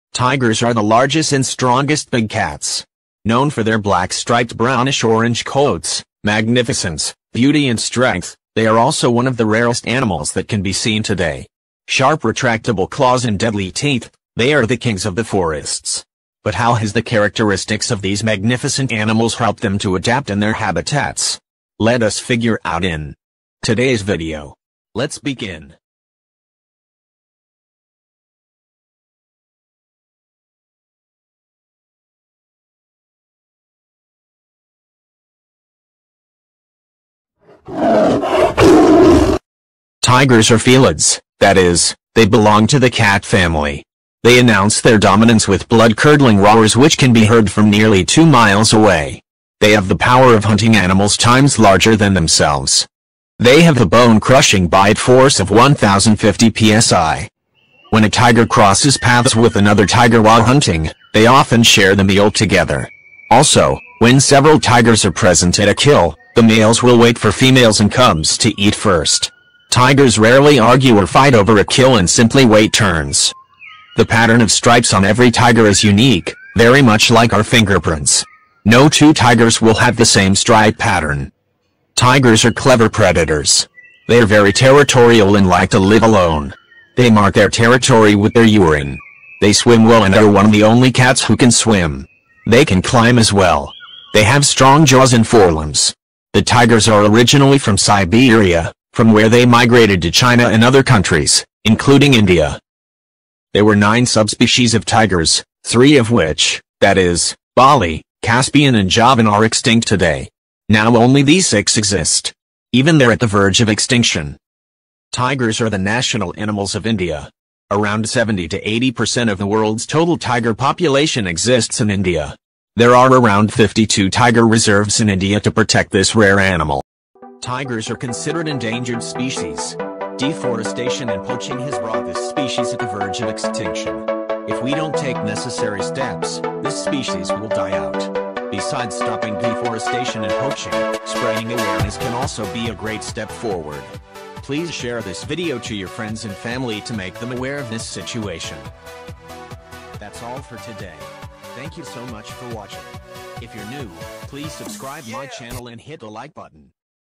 Tigers | Panthera Tigris | Roar of the Kings
Tigers are amazing animals. They are strong, majestic and fierce. But they're endangered too. If necessary steps are not taken, this species will die out. In this video, you can know about the current situation of theirs and how we can help them to survive.